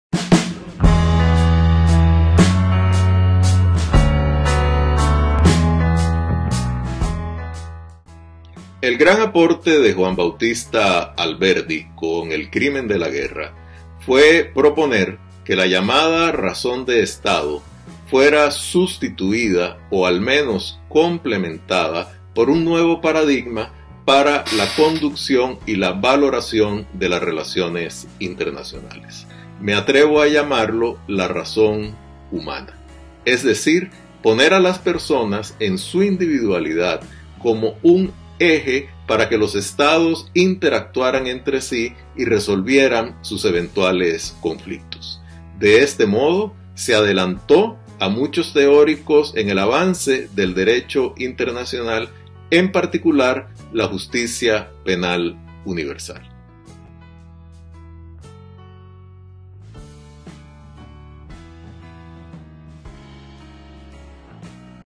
«The crime of war» by Juan Bautista Alberdi - Eduardo Ulibarri
Within the framework of the international day of peace, Eduardo Ulibarri, former Costa Rican ambassador to the UN and author of the prologue to Cadal's edition of the book "The crime of war", refers to the validity of this classic by Juan Bautista Alberdi.